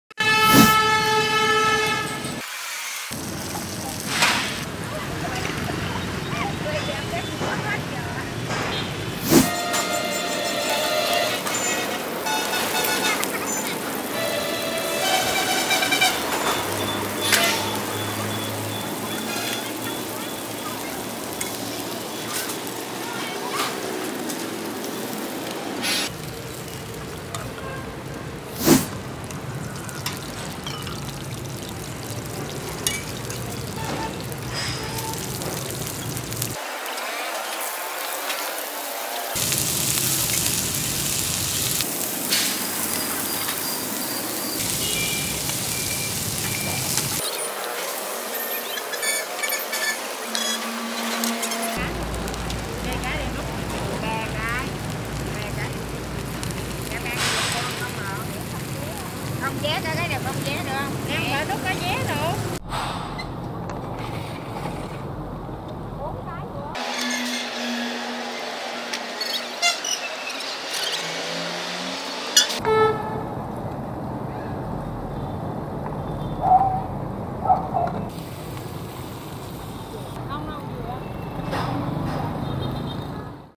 Bánh xèo tôm mực Nha Trang
Bánh xèo tôm mực Nha Trang có kích thước giống như chúng ta thường thấy bán ở vỉa hè Sài Gòn nhưng hấp dẫn và chất lượng hơn nhiều. Ngoài những thành phần bắt buộc, thì nguyên liệu chính để tạo nên “tên tuổi” cho món đặc sản này là tôm và mực ống tươi..